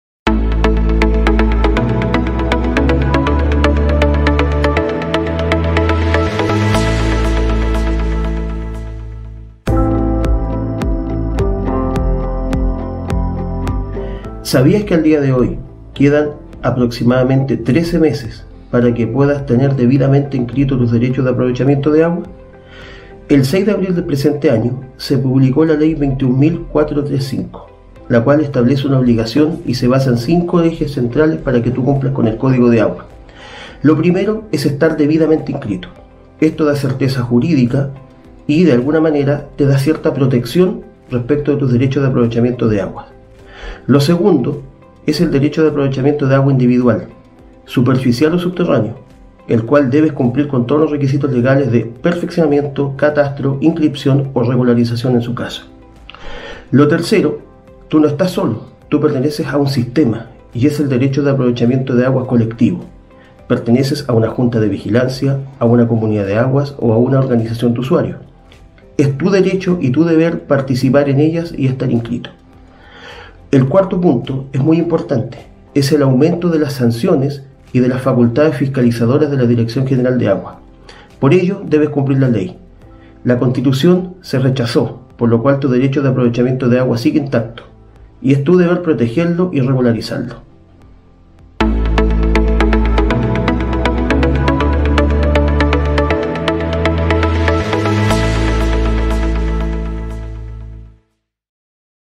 5 puntos claves para entender la reforma al Código de Aguas. Por el abogado Rodrigo Guerrero.
5 puntos claves para entender la reforma al Código de Aguas. Por Rodrigo Guerrero, abogado con postgrado en derecho empresarial, agua, minero y recursos naturales.